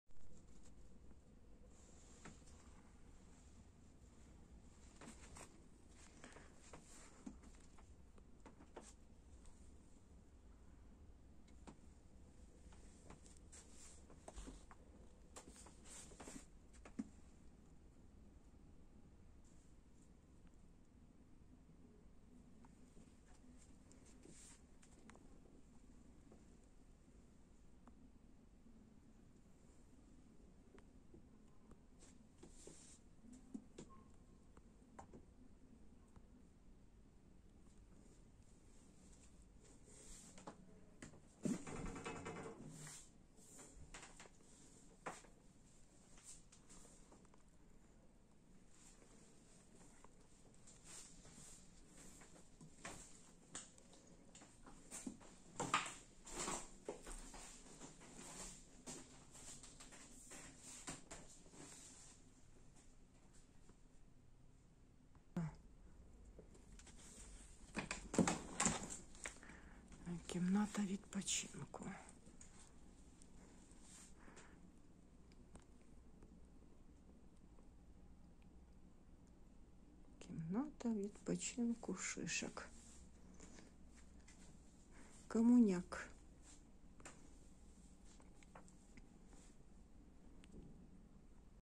МУЗЕЙ потягів минулого століття.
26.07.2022р. - приїзд Дамірчіку з літнього табору.